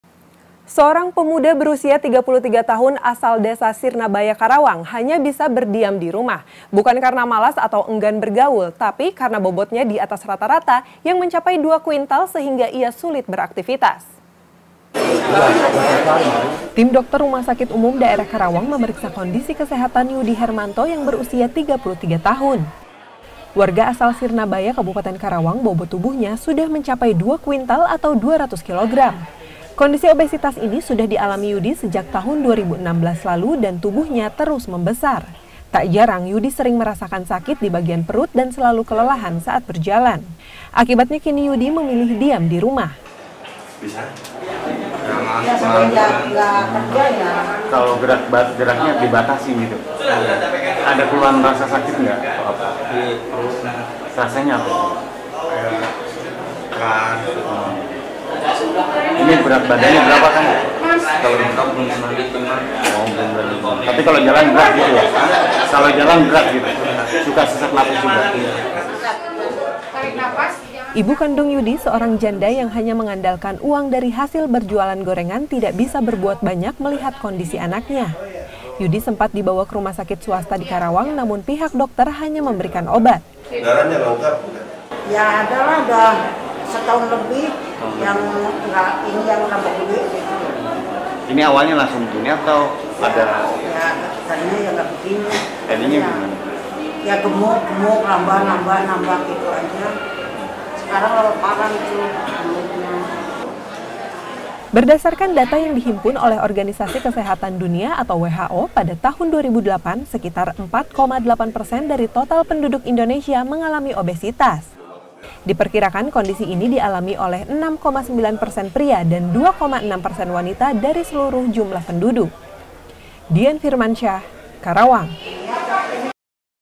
Pemuda 33 Tahun Berbobot 2 Kuintal Sulit Beraktivitas
Seorang pemuda berusia 33 tahun asal Desa Sinarbaya, Karawang, hanya bisa berdiam di rumah .

Like & Follow Facebook:
CNN Indonesia

cnnindonesia